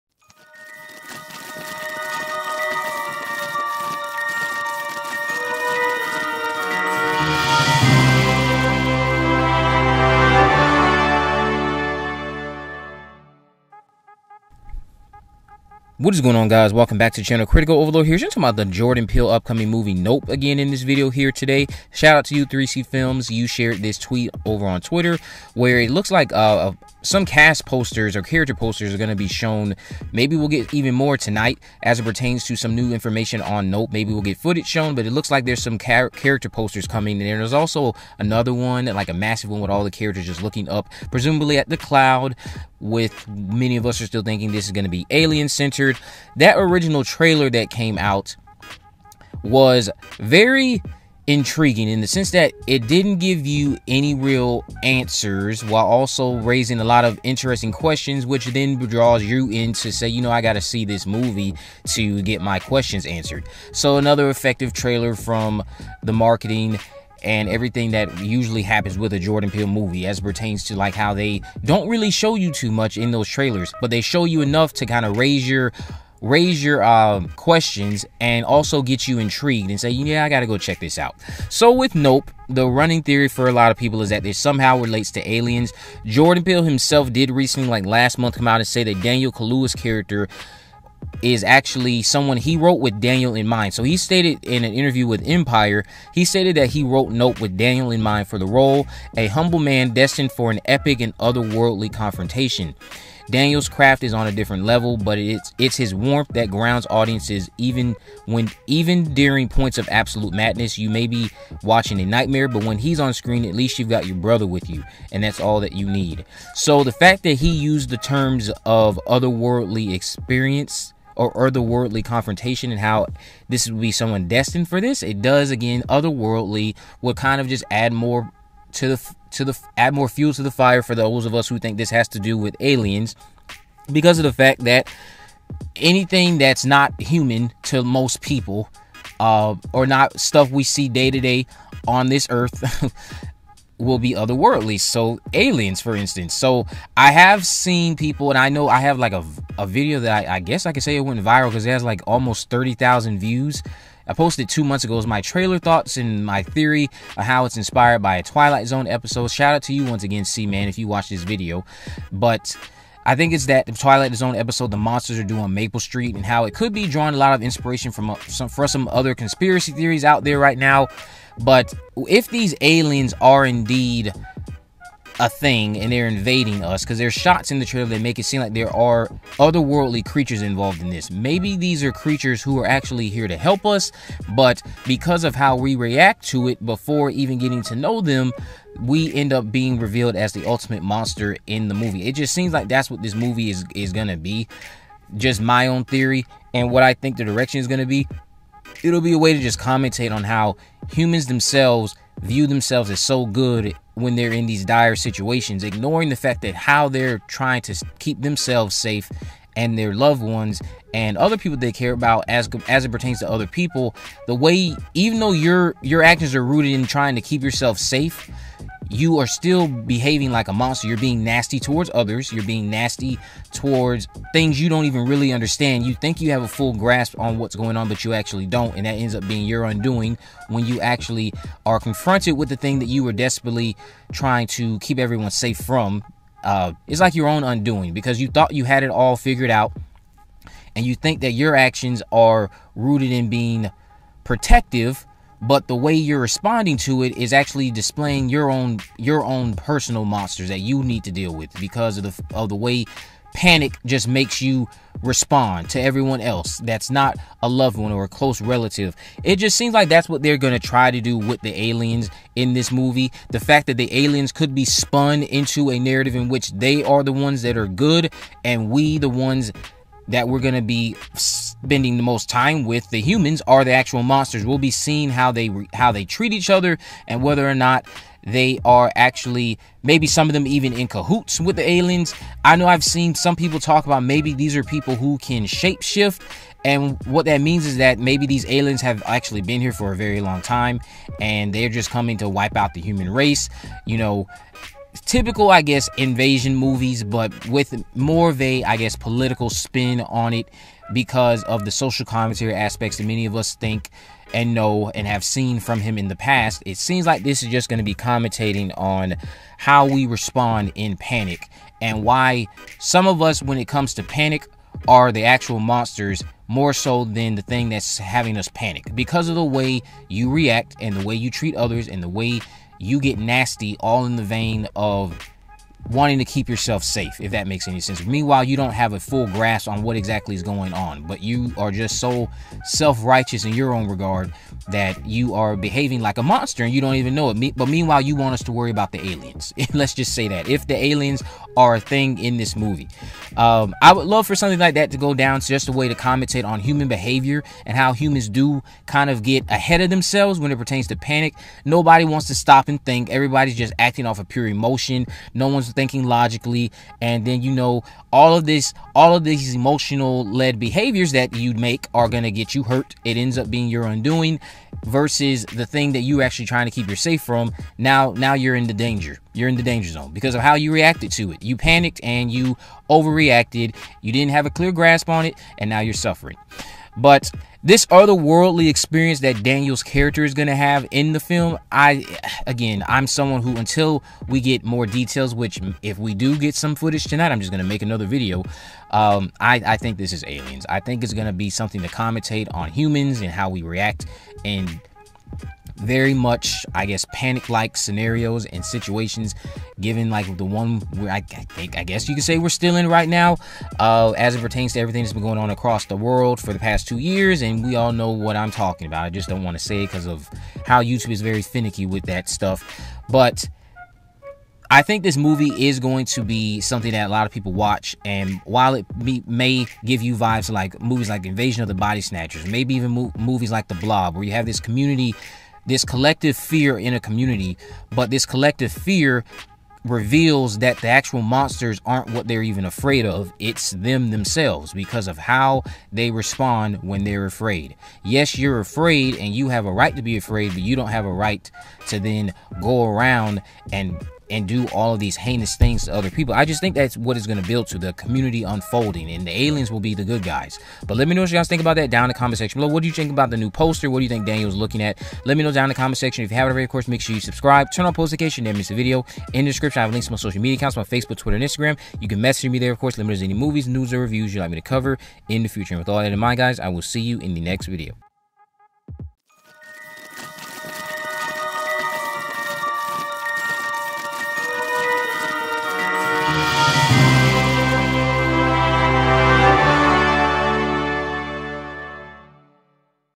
NOPE | NEW Poster + Jordan Peele Teased Alien Confrontation?
NOPE posters have been spotted at Cinema Con this week. Universal has its slate of film presentations later this evening, so new NOPE footage could be featured. Jordan Peele's upcoming third movie seems like it's an alien invasion narrative with a twist. Are the aliens actually here to help humans but we don't allow them to because of our poor reactions in moments of panic?